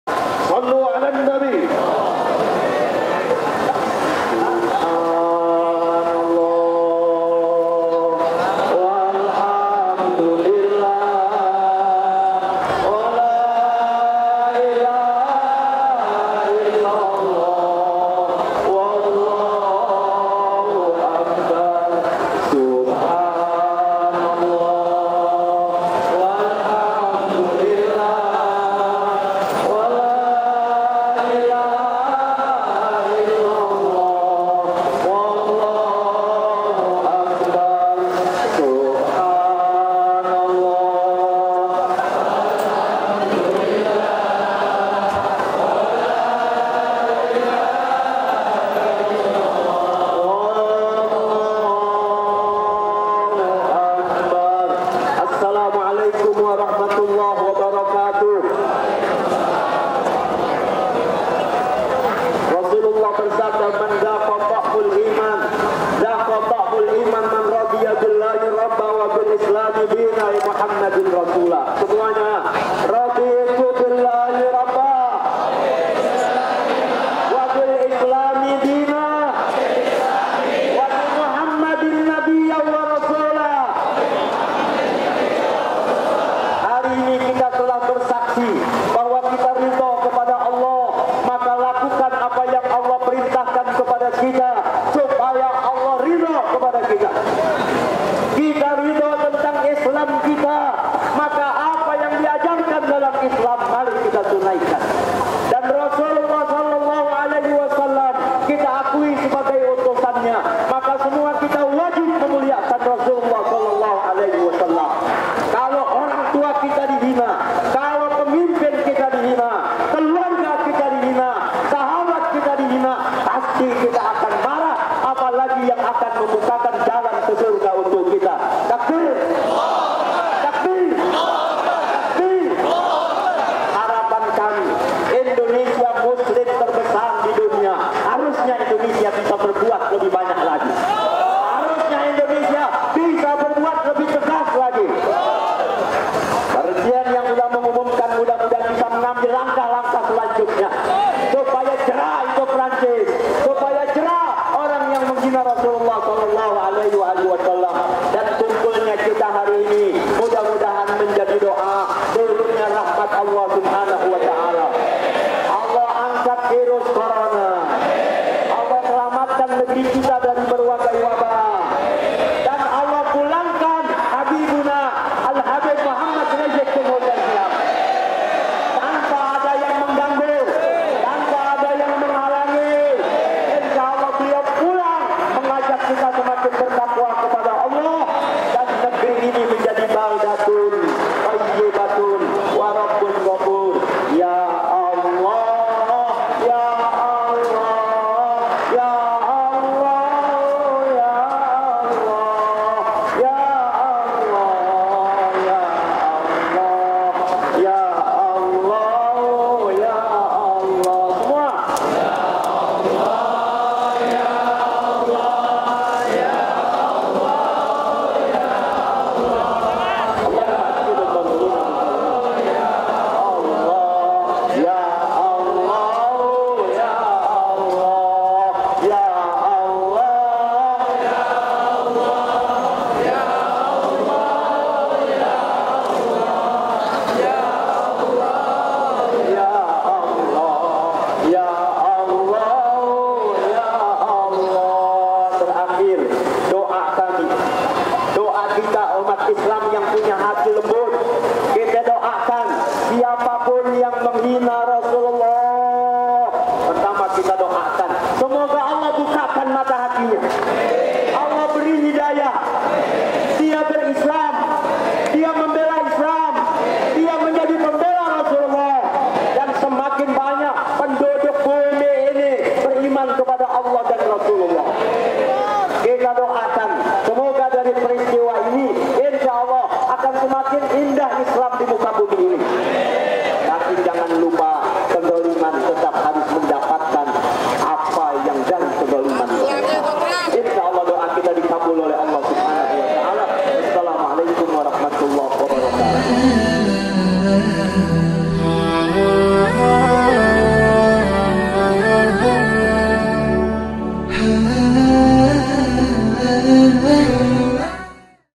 Orasi Aksi Bela Nabi Di Depan Kedubes Perancis, Kita Bela Kemuliaan Rasulullah | Ustadz Abdul Syukur
🎥 Kita Bela Kemuliaan Rasulullah | Ustadz Abdul Syukur